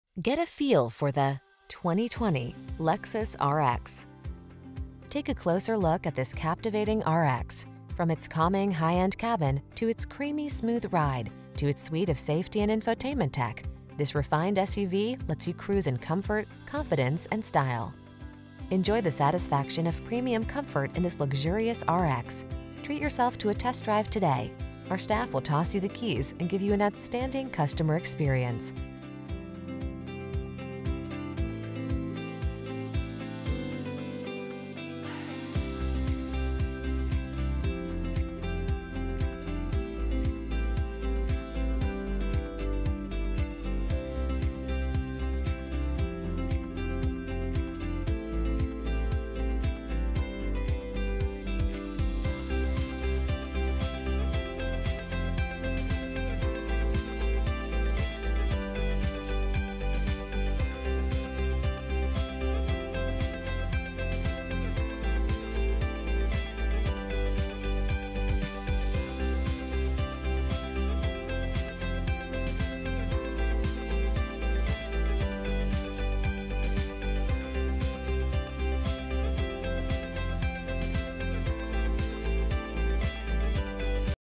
2020 Lexus RX Nashville, TN 213095
Obsidian  2020 Lexus RX available in Nashville, Tennessee at Motorcars of Nashville Mt. Juliet.
2020 Lexus RX RX 350 F SPORT AWD - Stock#: 213095 - VIN#: 2T2SZMDA6LC213095

Motorcars of Nashville - Mt Juliet
203 Athens Drive